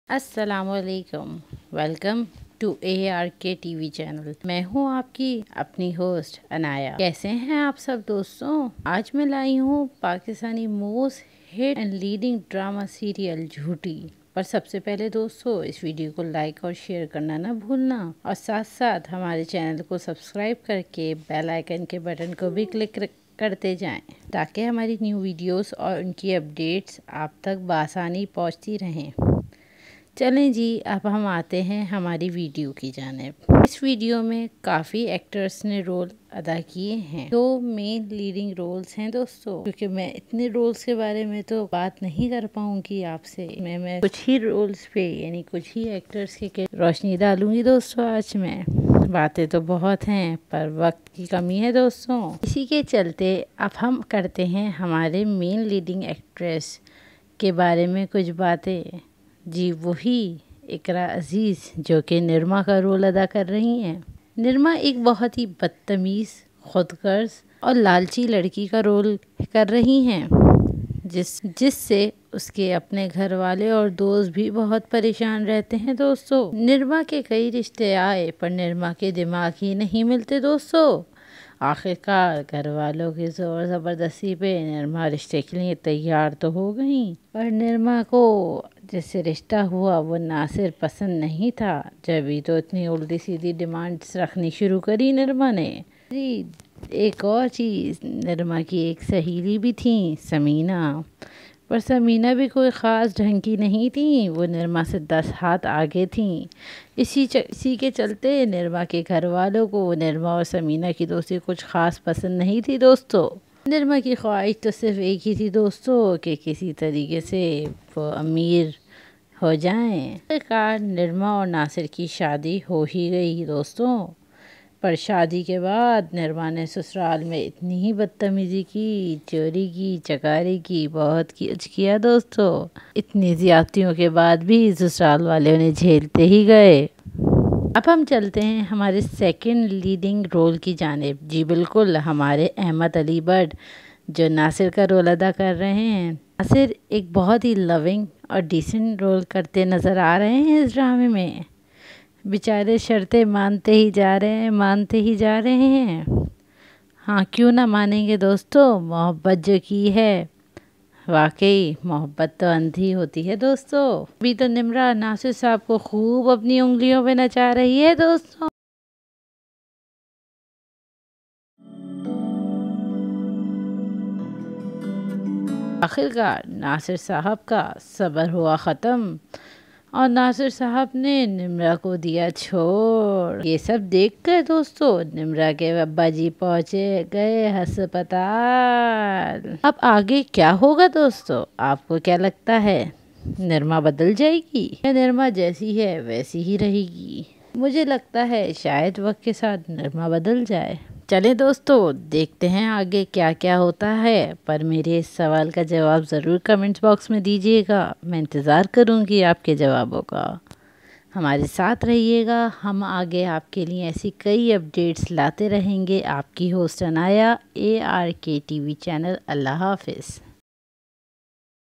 Jhooti ||Drama Review|| (ARK TV)
As per the revealed teasers Iqra Aziz is shown a materialistic daughter of a Shopkeeper who dreams of marrying someone atleast not a shopkeeper and preferably someone rich but unfortunately Ahmed Ali Butt, a cute & innocent character falls in love with her & sends her a marriage proposal and despite her opposition they both get married. Now, comes the twists where Iqra starts playing games , against Ahmed and make false allegations against him. Later, it is expected that she will fall in love with Yasir Hussain, presumably a richer guy.